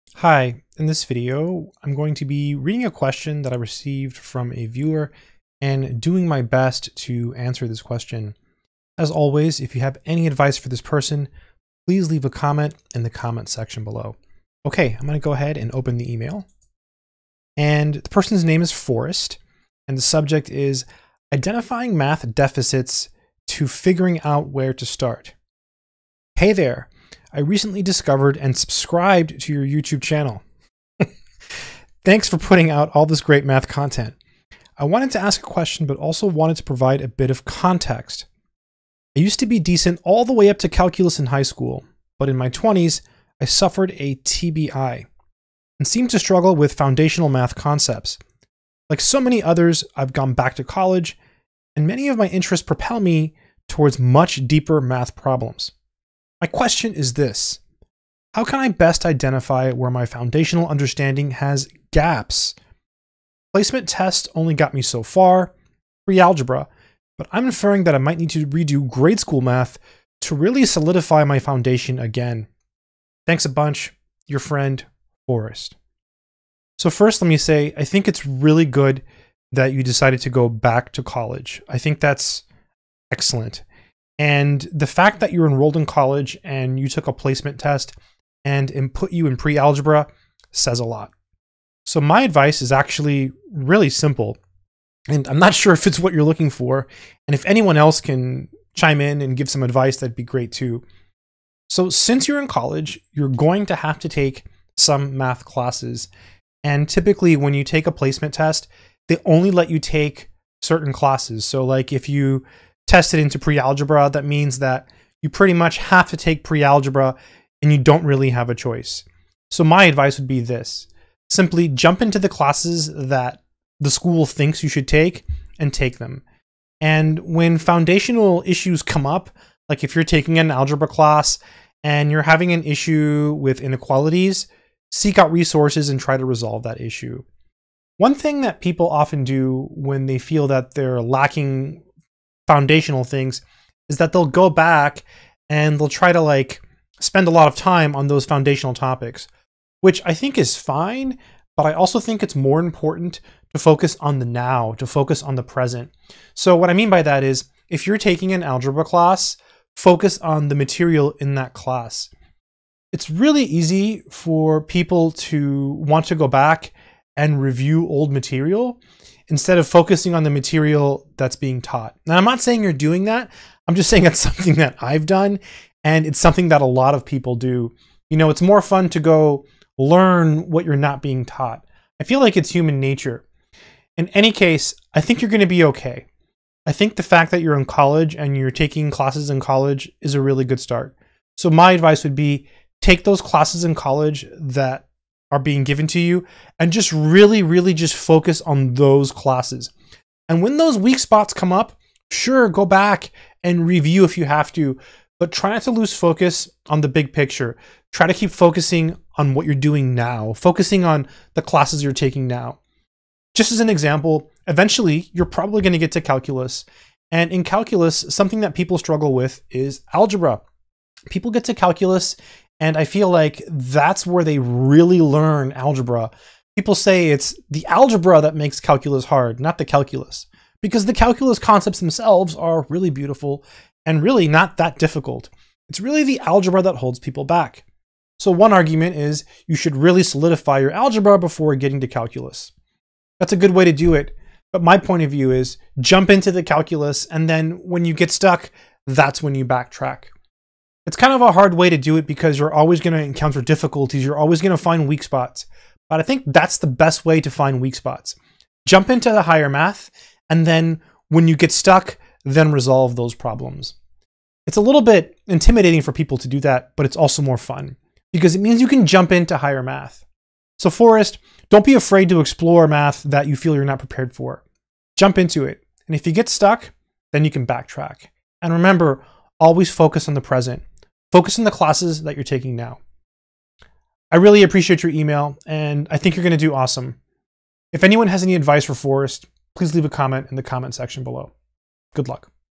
Filling in Gaps in Math Knowledge
In this video I answer a question I received from a viewer. He is going back to college and is trying to fill in gaps in his math knowledge. Do you have any advice?